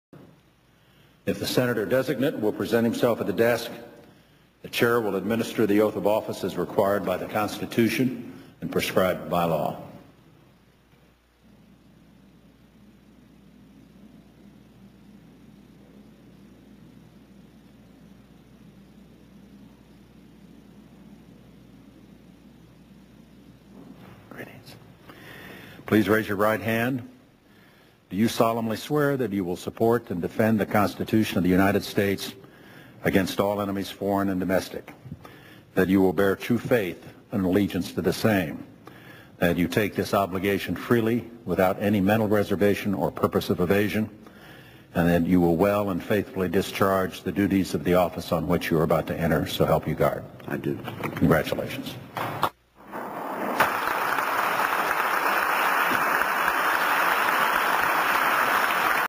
Senator Wicker's Swearing In Ceremony
Vice President Dick Cheney swears in Senator Roger Wicker as the new United States Senator from Mississippi.